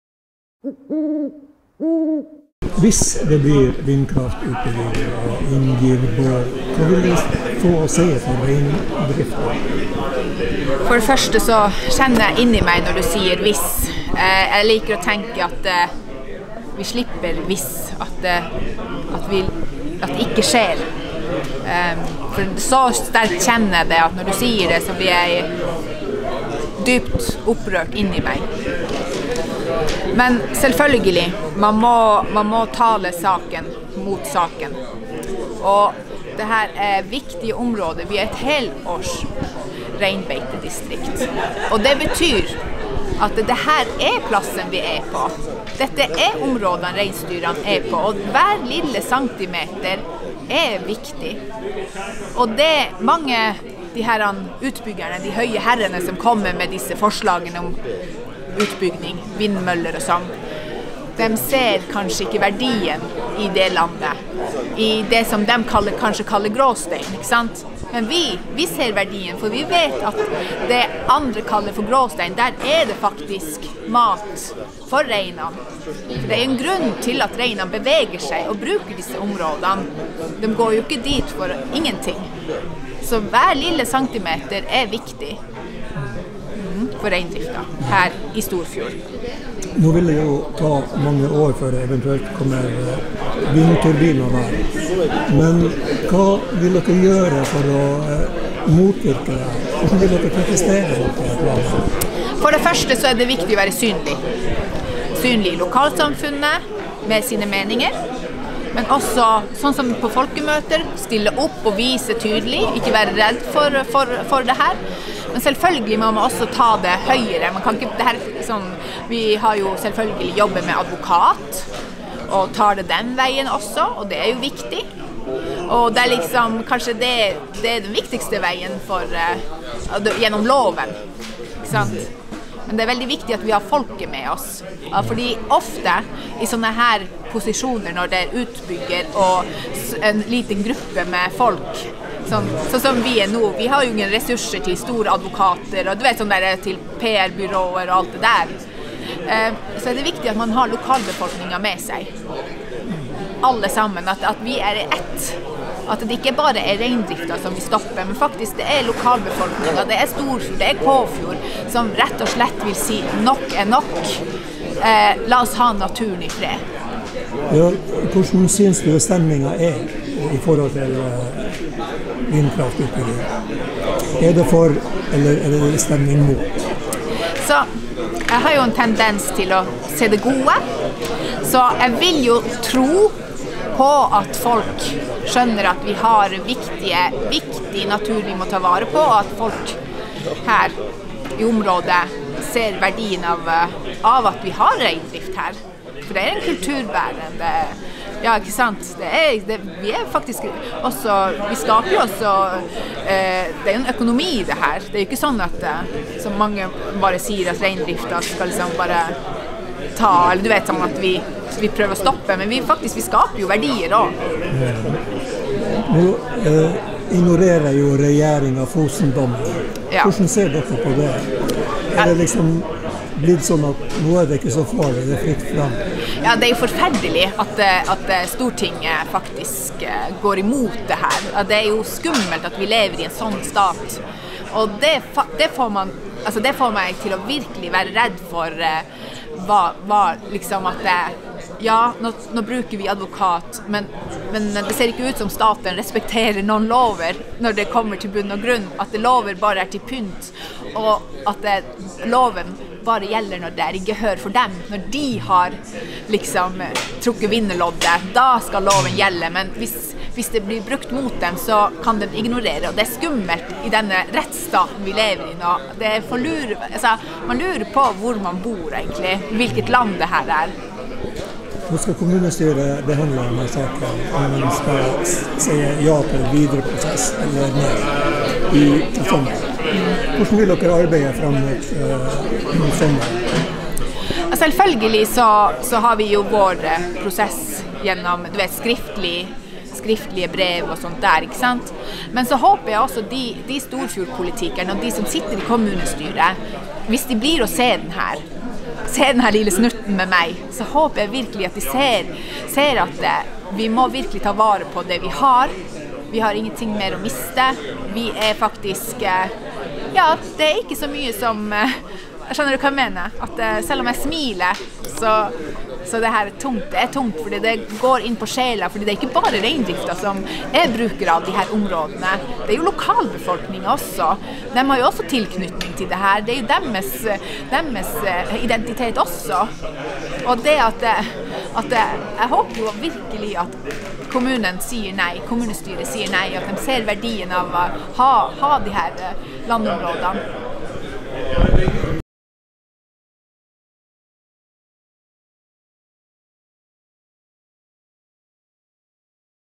Ingir Båhl
Ingir Båhl fra Helligskogen reinbeitedistrikt forteller om hvilke følger et vindkraftverk vil få næringa og for naturen.